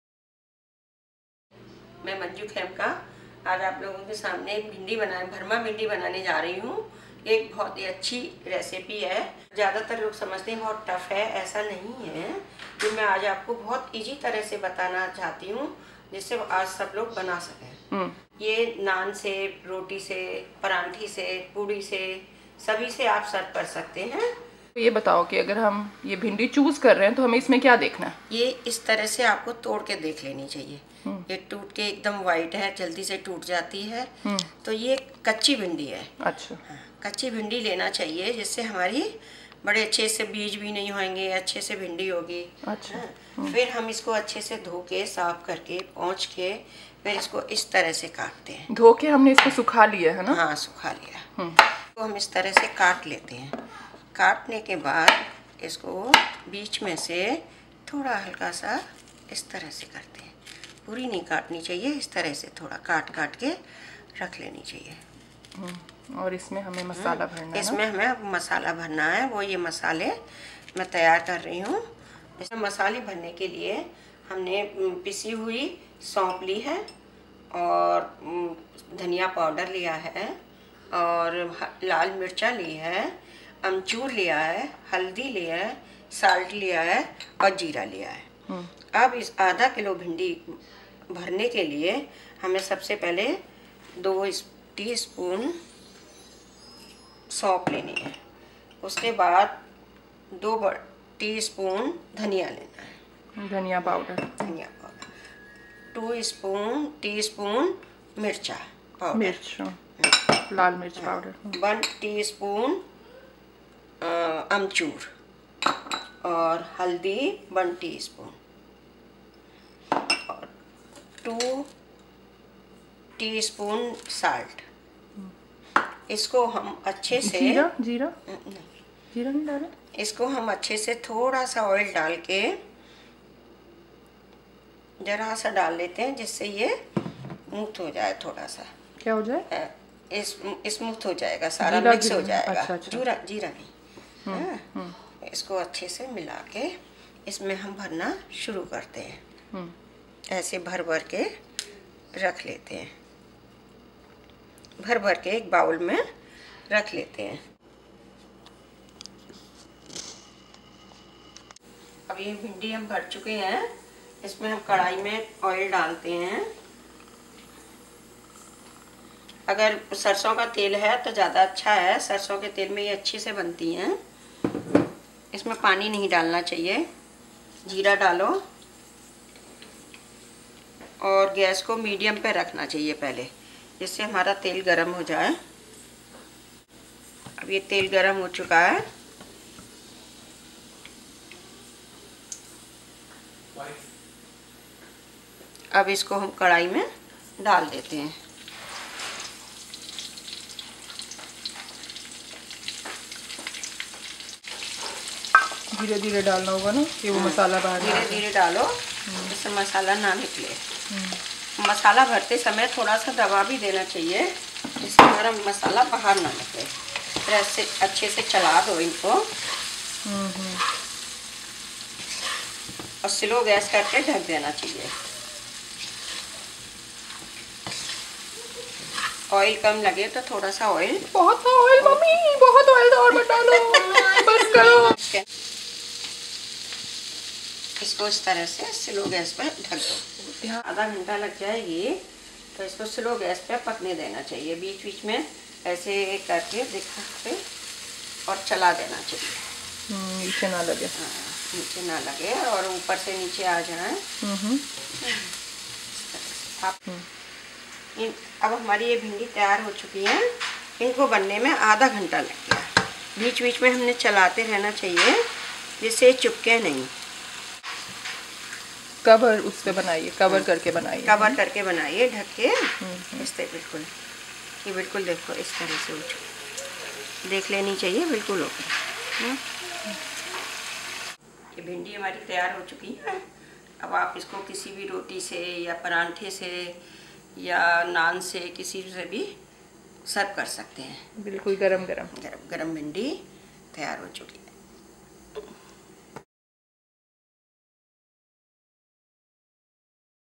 Bhindi Bharwaan - by Manju Khemka
Spicy ,flavorful bharwan bhindi to complement roti , parantha , naan!Simple to follow recipe by my mother in law - Mrs Manju Khemka . Absolutely delicious and fantabulous :)